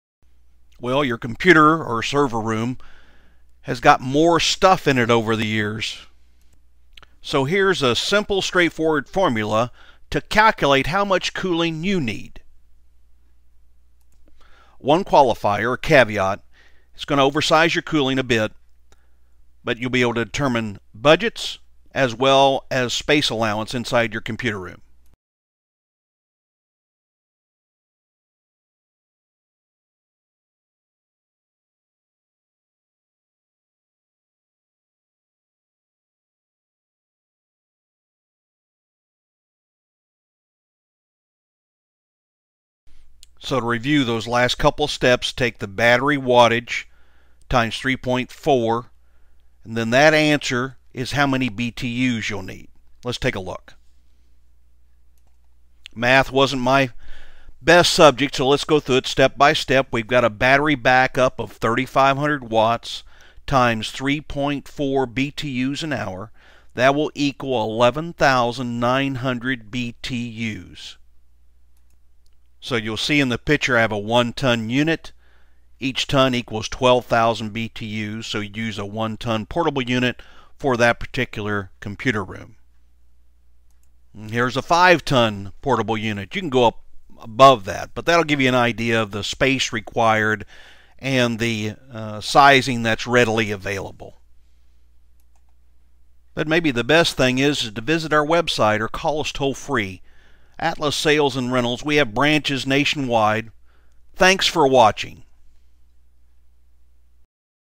How to Calculate Cooling Needed for Your Server Room
Learn how to size the proper portable cooling unit for your server room. This short video will teach you how to calculate the size needed using portable AC units.